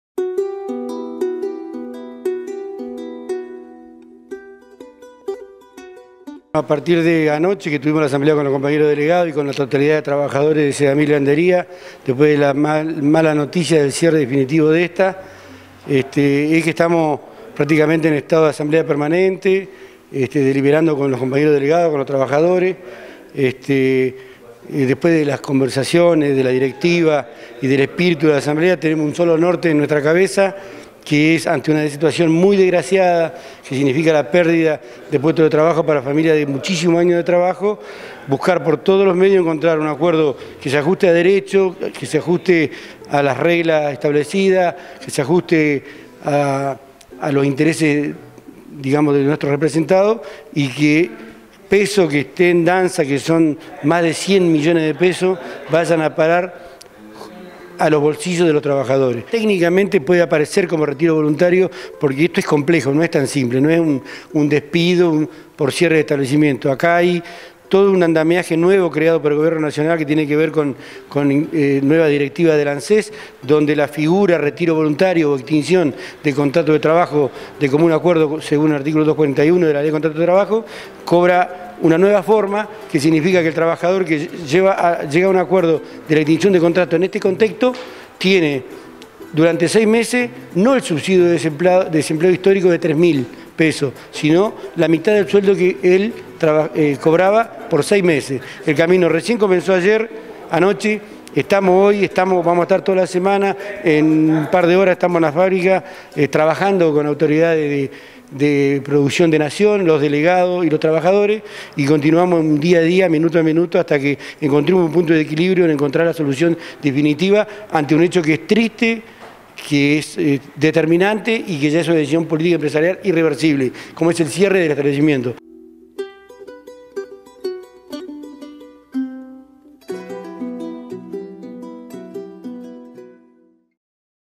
El drama de los hilanderos
Los trabajadores despedidos se encuentran desde ayer en asamblea permanente. El objetivo es conseguir el mejor arreglo posible para los trabajadores.
Sergio Cárdenas, Secretario Adjunto de AOT, dice que hay dinero dando vueltas y pretende que todo vaya para los trabajadores. Dice que el camino recién comienza.
12 Noticias